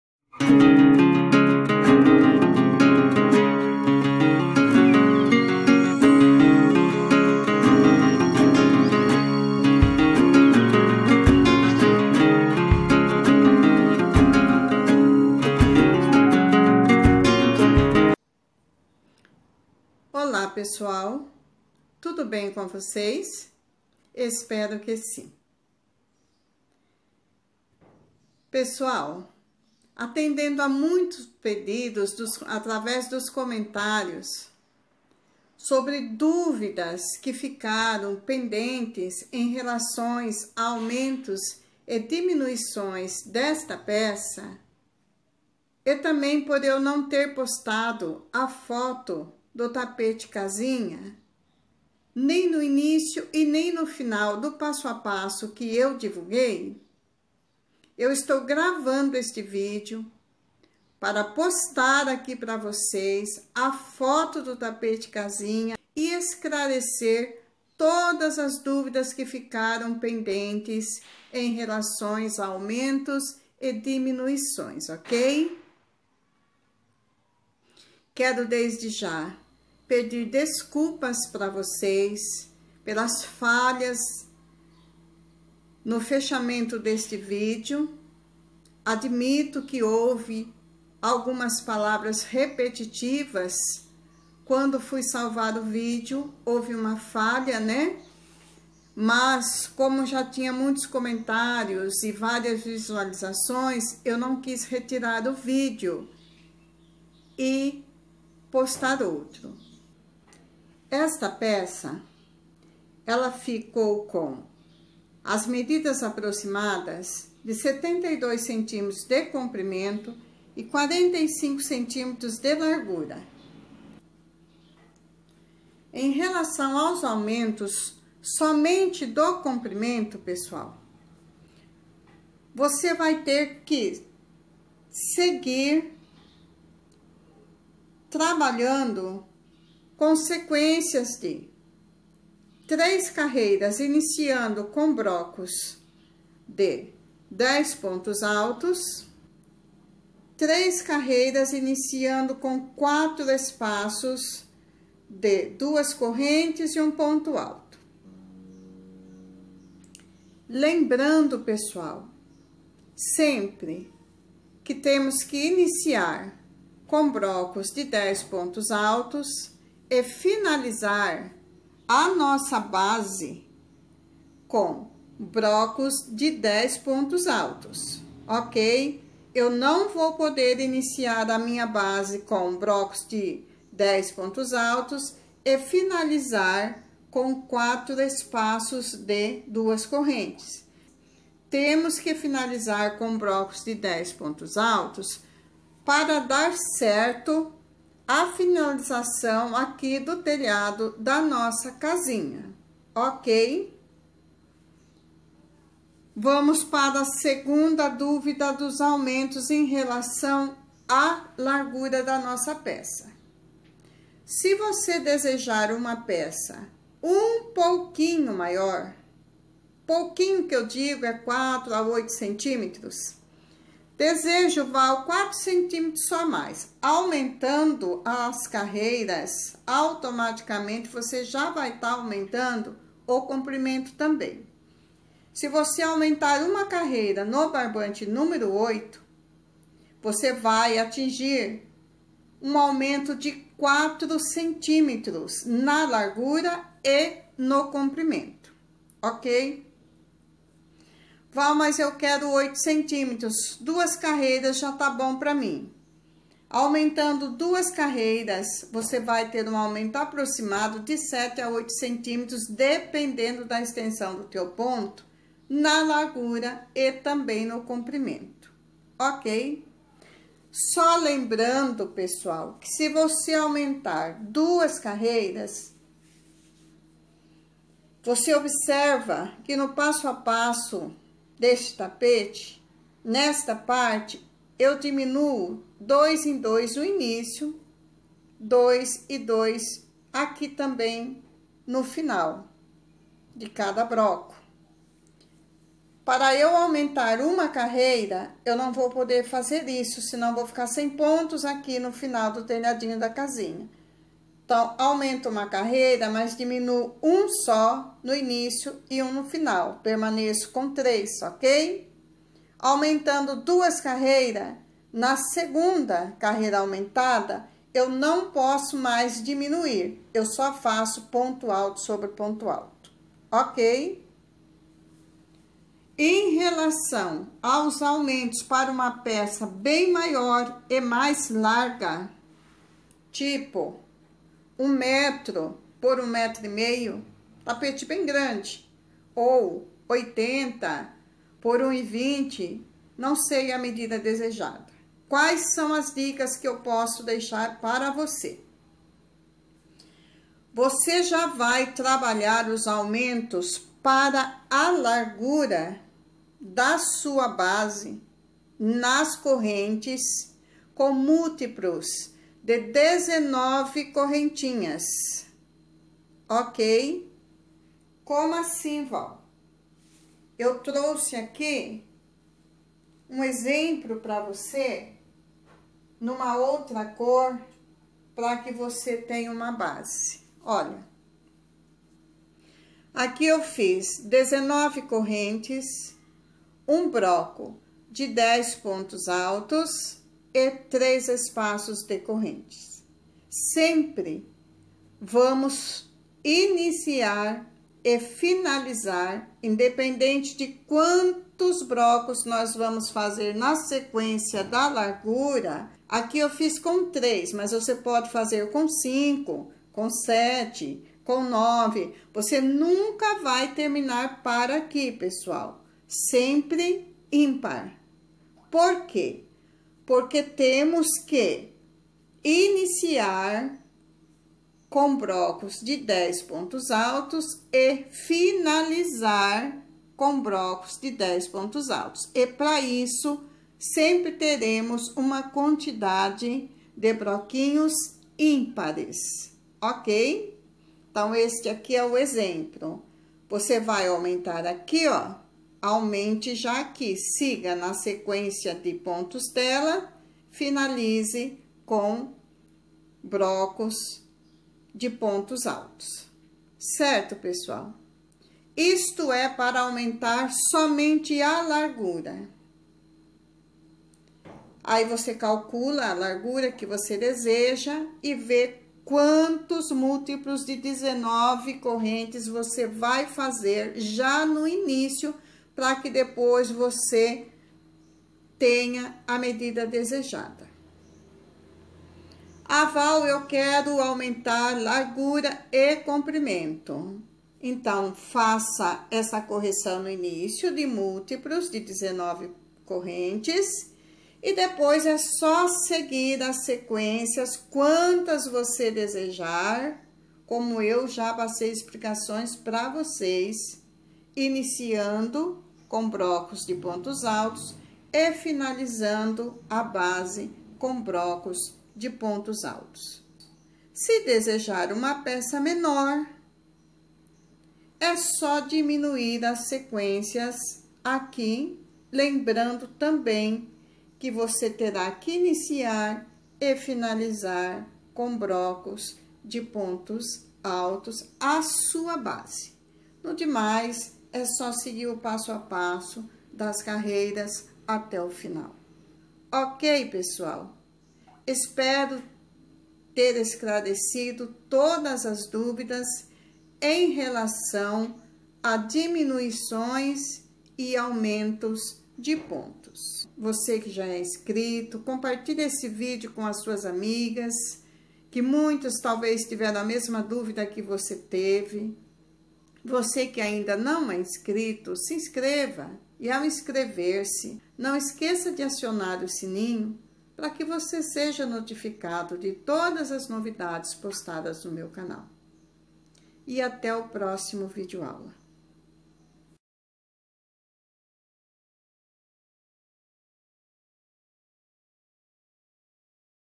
TAPETE CASINHA- ( DICAS PARA FAZER OS AUMENTOS)
VOCE TEM DUVIDAS? DE COMO AUMENTAR ESSE TAPETE
 ASSISTA  ESSE VÍDEO E TIRE TODAS AGORA MESMO.